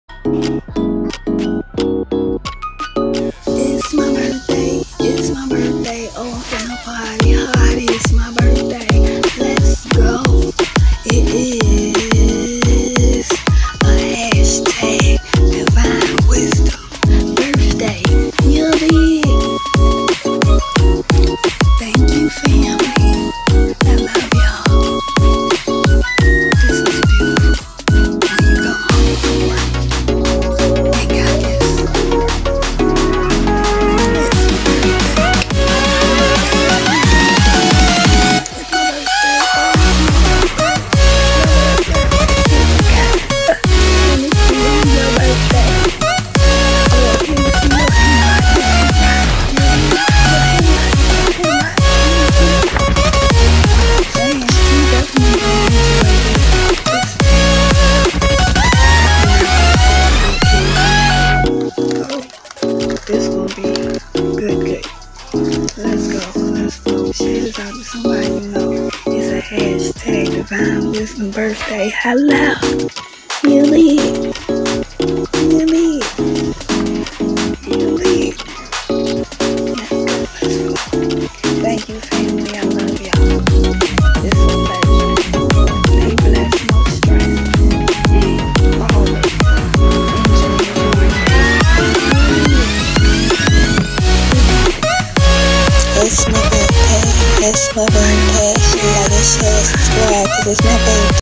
IT'S AN @DVINEWIZDOMTruthspitt BIRTHDAY 🎂
IT'S MY BIRTHDAY WE GOT TO PARTY HARDY

@DVINEWIZDOMTruthspitt  BIRTHDAY 🎂 COME ON

IN ENJOY HAVE A SLICE OF A CHOCOLATE

CANDY APPLE IT'S A PLEASURE TO

CELEBRATE 🎉 MY BORNDAY TY GOD ALSO

FAMILY I APPRECIATE THE LOVE YOU ALL

POURED ON ME TY TY TY FAMILY!!!